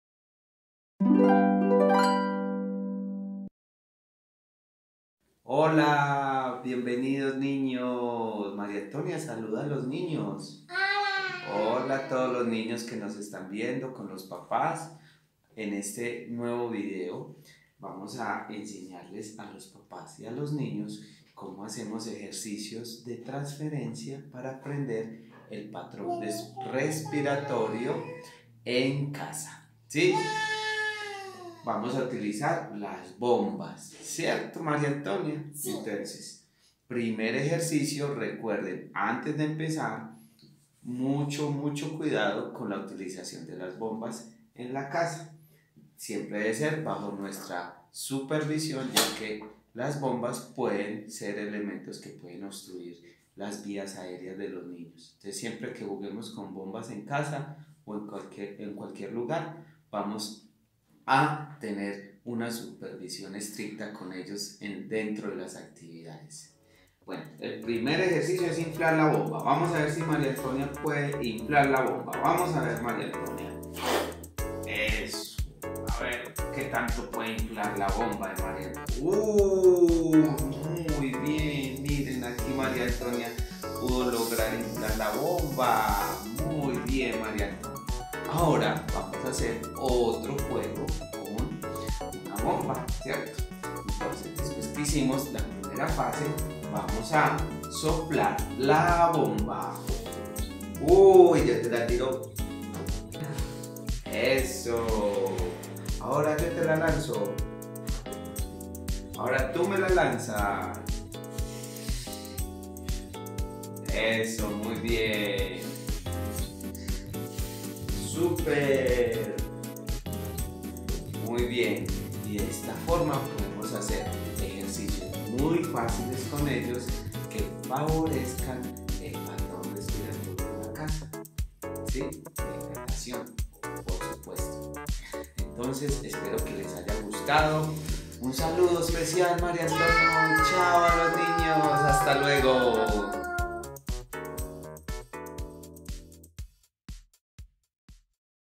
Ejercicios en casa para la respiración en natación.
Ejercicios para favorecer y acompañar el patrón respiratorio en natación.